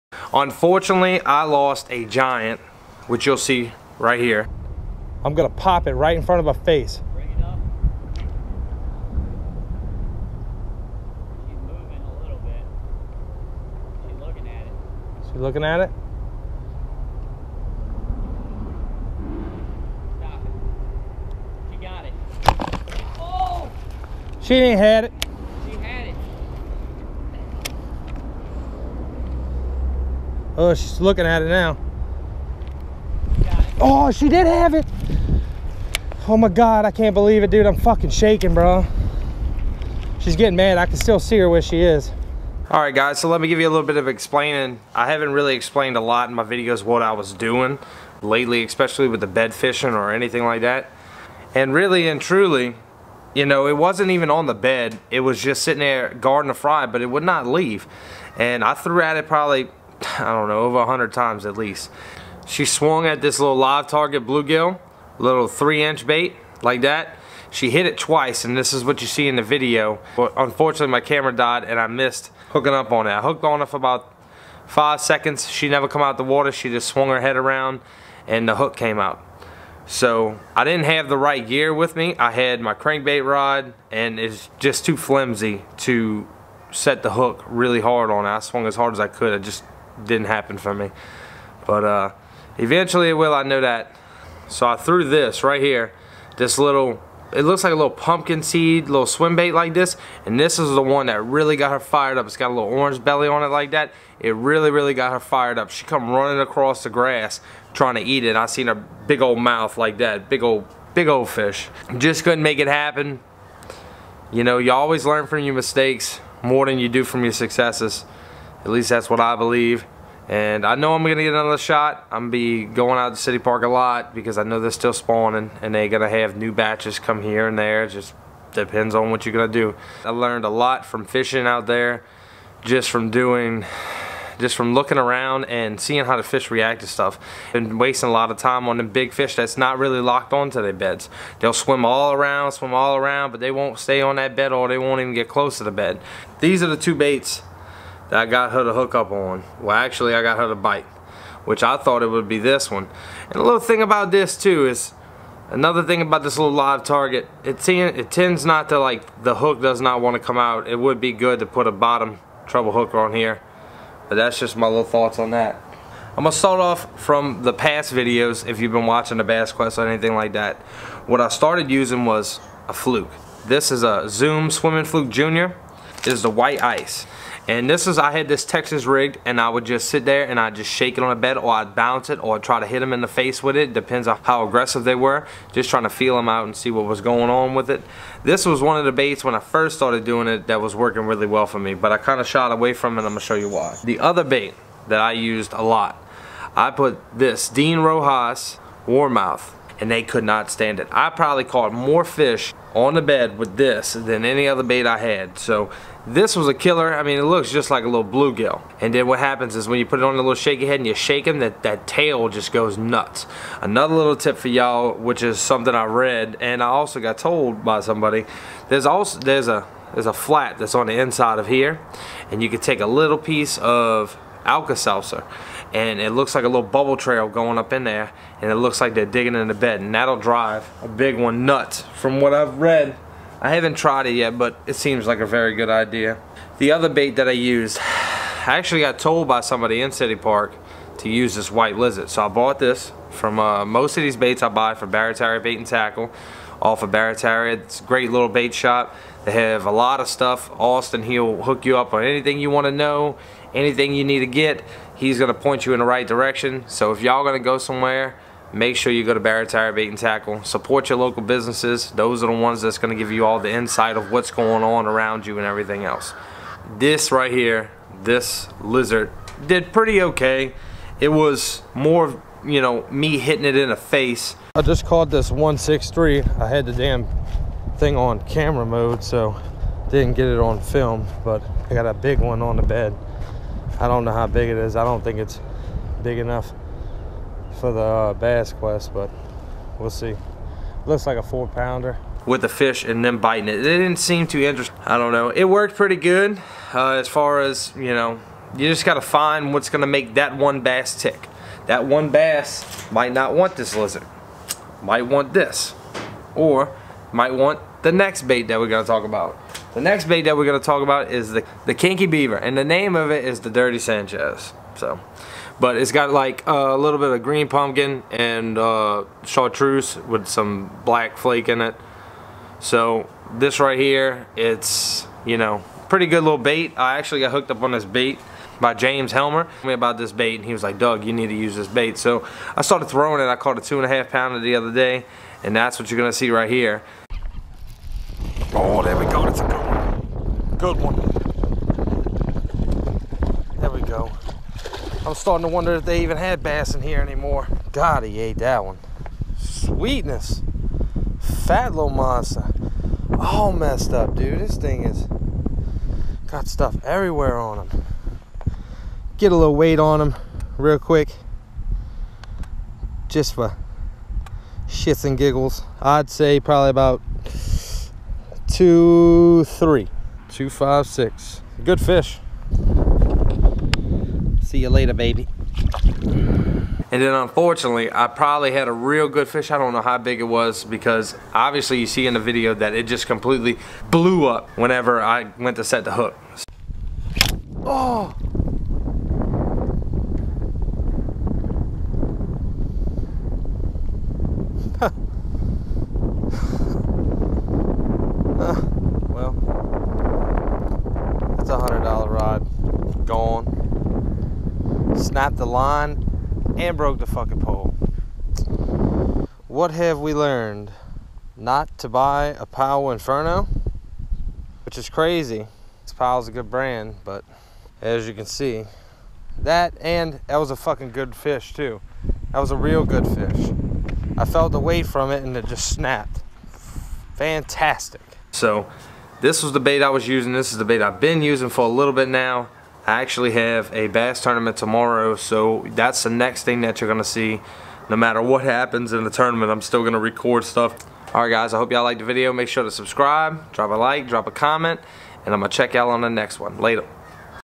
Giant Bass Gone,Broken rods,And Tips On Bed Fishing
Tips on how I've been catching bed fish in city park.. thanks for all the tips from you guys..Make sure you go to barataria bait and tackle link is